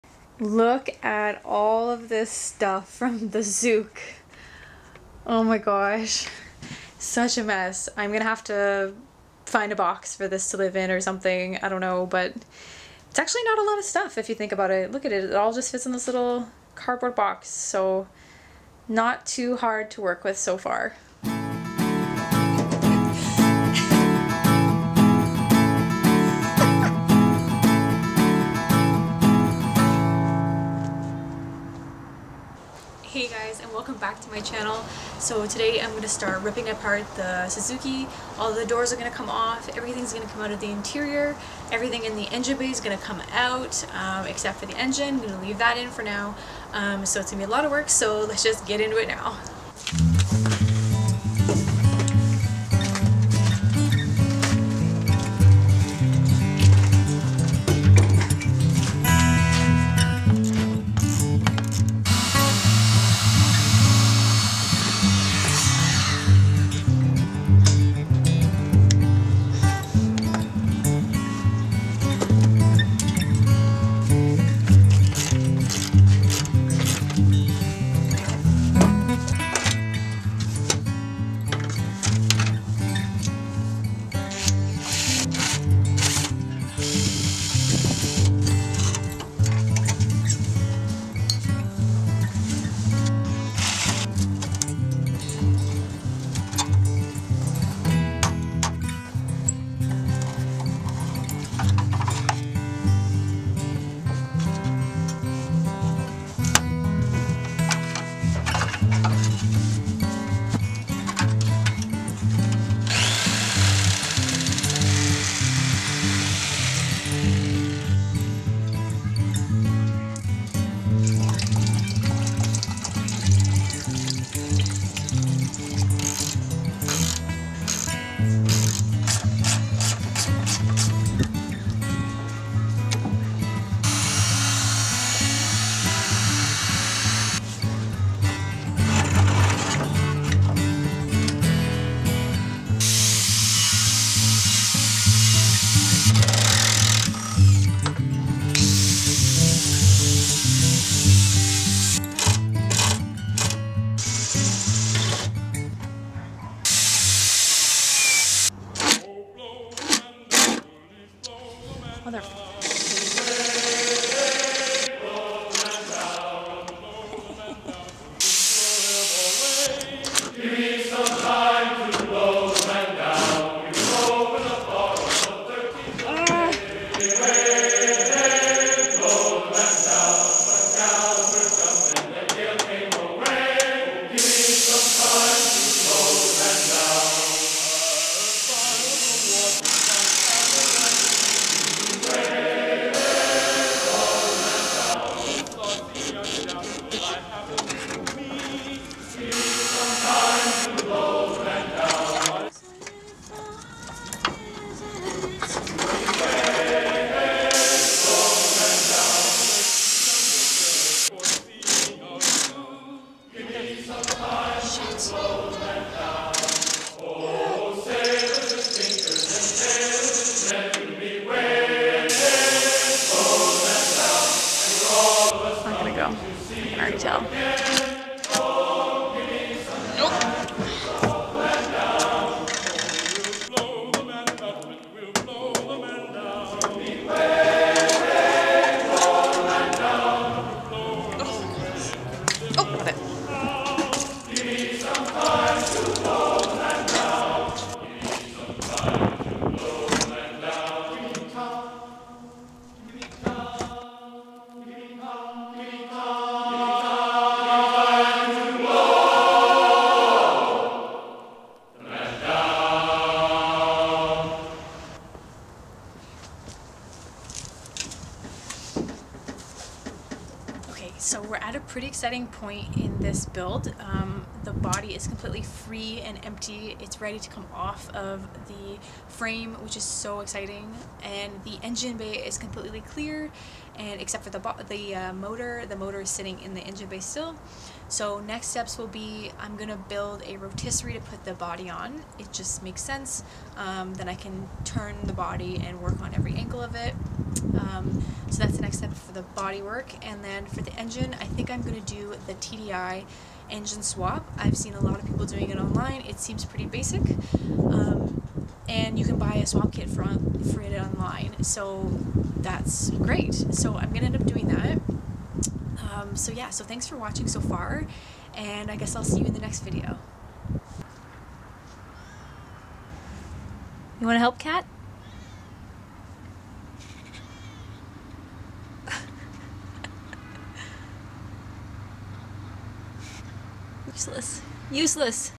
Operation Samurai Part 3 - Disassembling My Suzuki Samurai
A long week or so of working, and finally I have most of the guts out of my Samurai. There is nothing like working with your hands, testing your patience, playing with tools, and spending time outdoors!